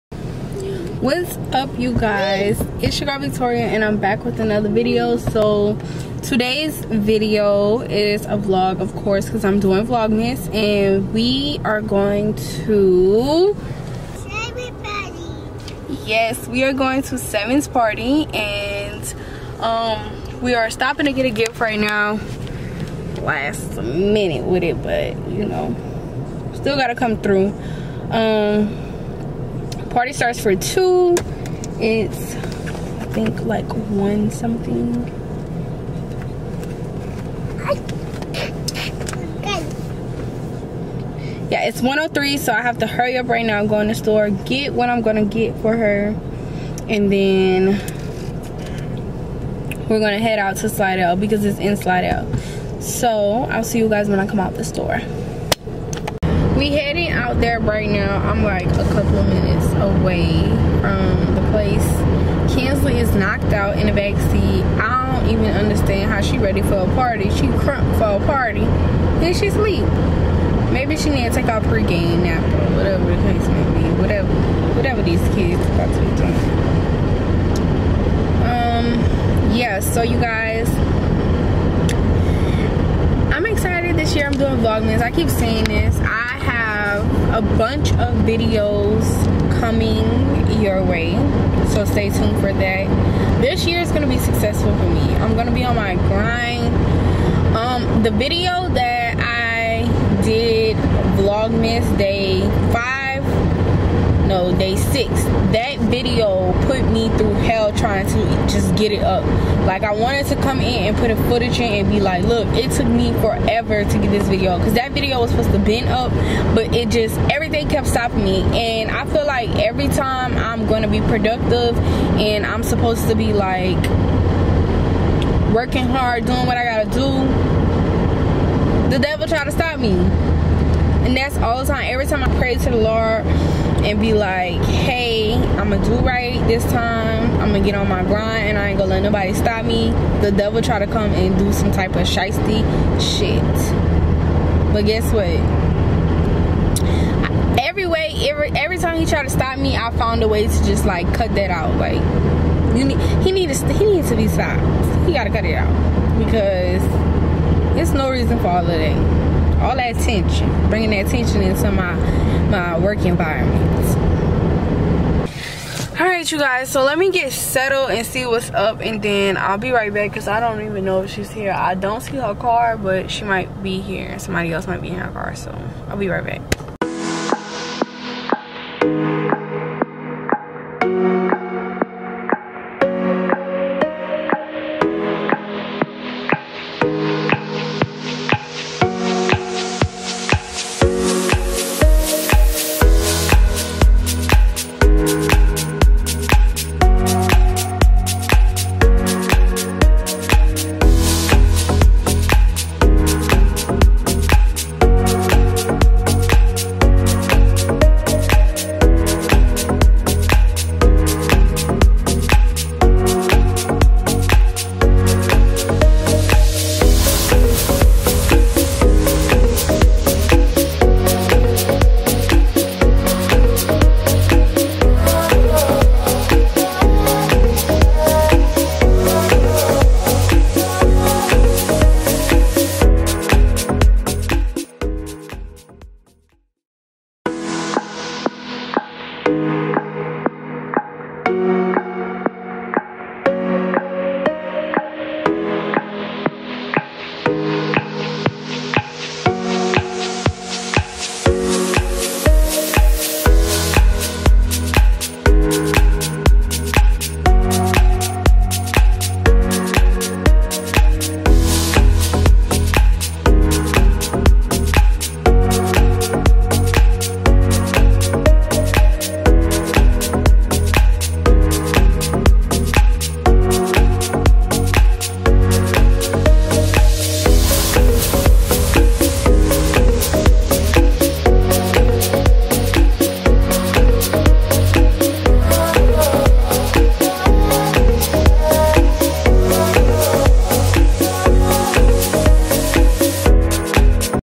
VLOGMAS 7: WE SEEN PRINCESS TIANA , BRINGING KINSLEE TO A BIRTHDAY PARTY
HELLO EVERYONE👋🏽

Today I will be taking Kinslee to one of her friends birthday party. She is very excited to celebrate with her friend. There was also a special guest. Princess Tiana from the Waltdisney movie The princess and the frog. They enjoyed her very much.